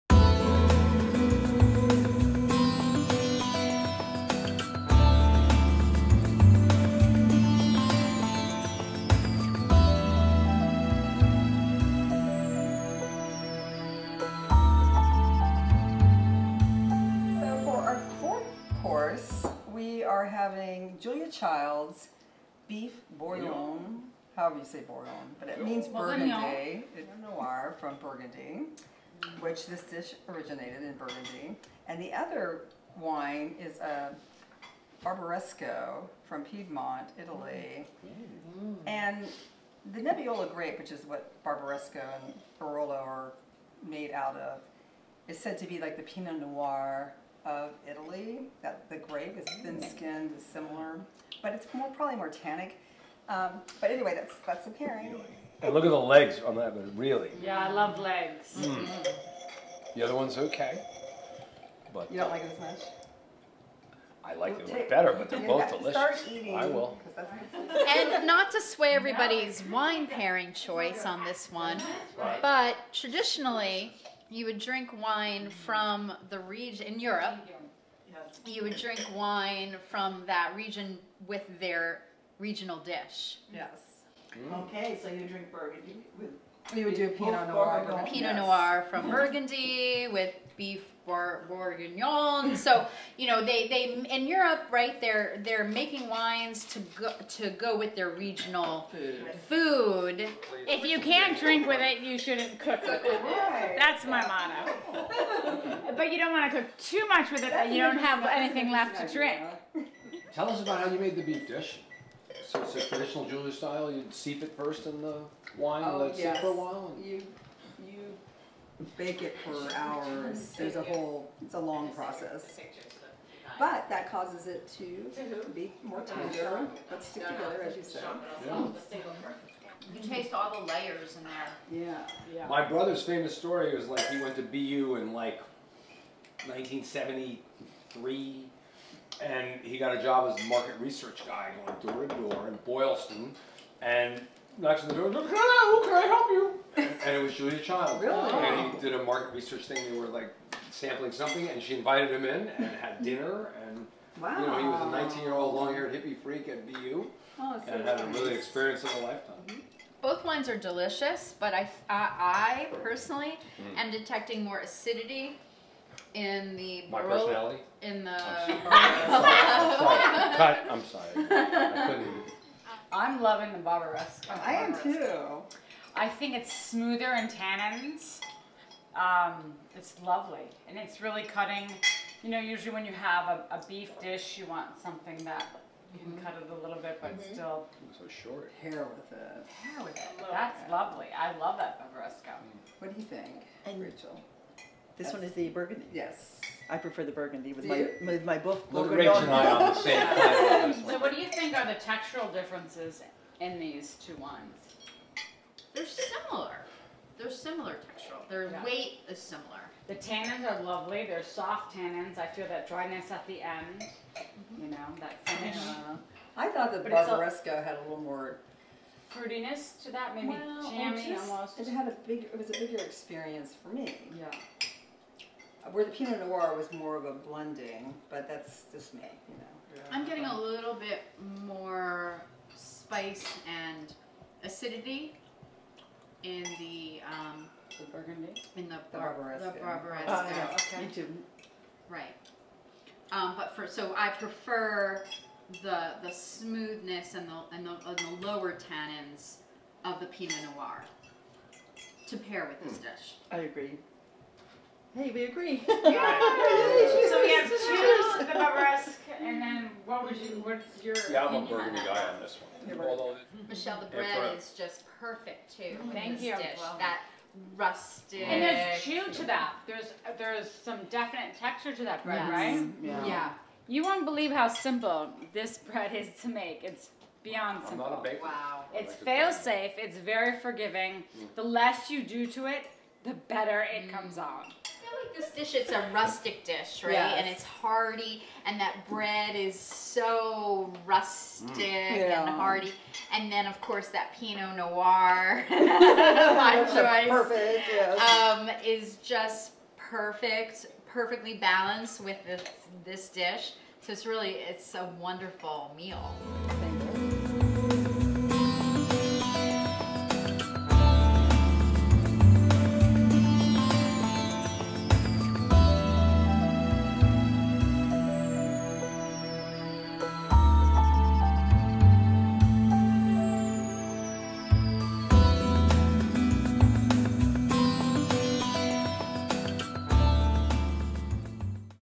"A Bountiful Feast" Entrée Course Pairing
"A Bountiful Feast" Entrée Course Pairing! Learn what the perfect pairing with a braised beef dish is! These red wines are luscious and paired with Julia Child's Beef Bourguignon recipe bring it to a whole new experience! Can find this recipe at libationsandfood.com and my very own beef bourguignon recipe